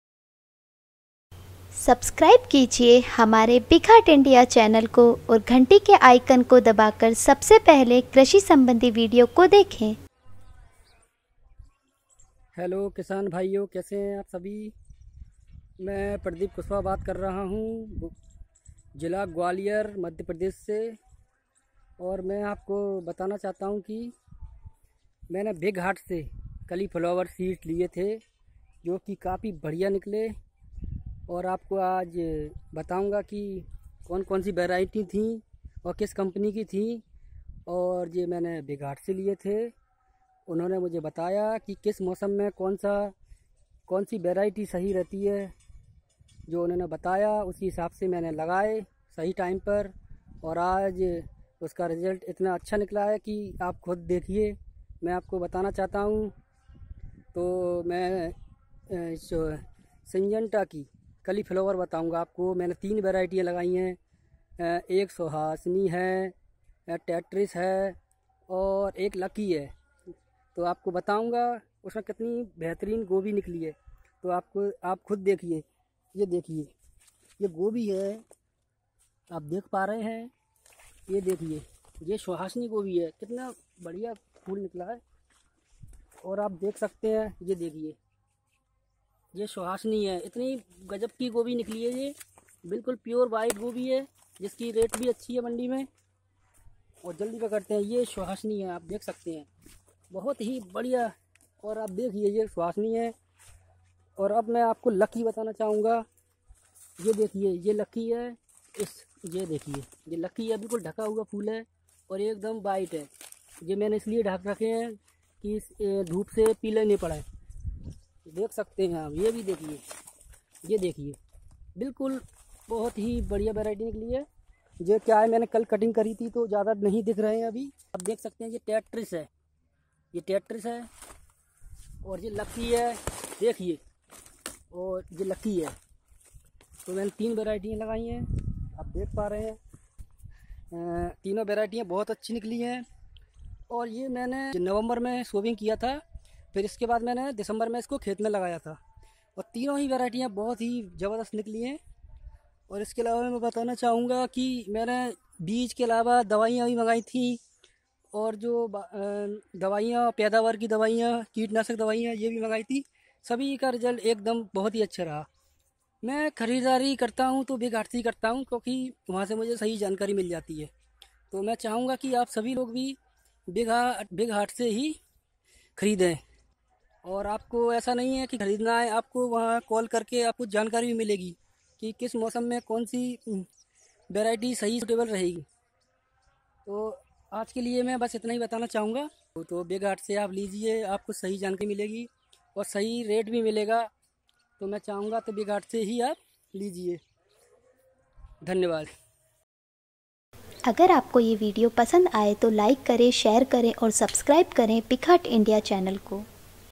Cauliflower Cultivation | M.P. Farmer's BigHaat Experience
Gwalior, Madhya Pradesh, Cauliflower Cultivating Farmer is explaining his experience with BigHaat and its services & digital platform for Agriculture and how it helped him to know about best cultivation practices on cauliflower crop.